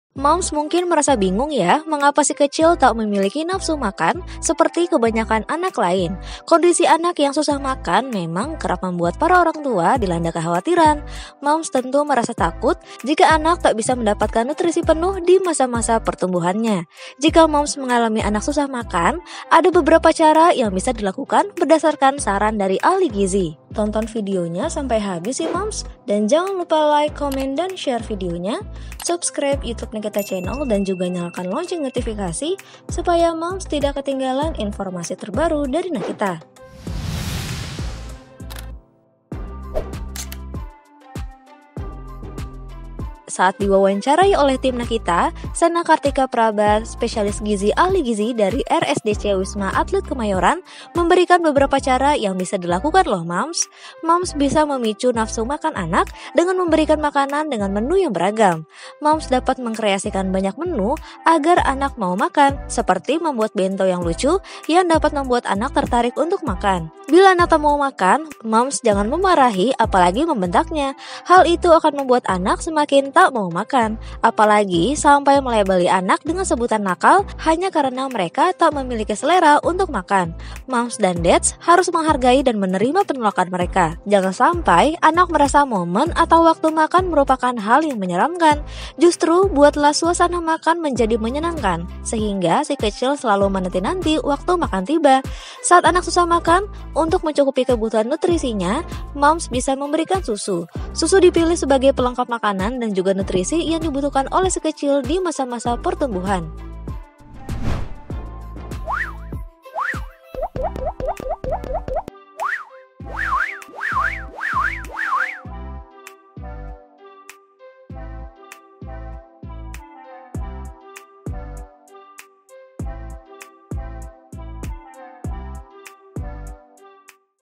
Hanya dengan Cara Ini, Moms Jadi Gampang Ajak Si Kecil untuk Makan Lho!
Moms mungkin merasa bingung ya mengapa Si Kecil tak memiliki nafsu makan seperti kebanyakan anak lain. Kondisi anak yang susah makan memang kerap membuat para orangtua dilanda kekhawatiran. Moms tentu merasa takut jika anak susah mereka tak bisa mendapatkan nutrisi penuh di masa-masa pertumbuhannya. Jika Moms mengalami anak susah makan, ada beberapa cara yang bisa dilakukan berdasarkan saran dari ahli gizi.
00:00 Intro
00:43 Ahli Gizi Menyarankan Hal Ini Agar Nafsu Makan Anak Meningkat

ADVERTISING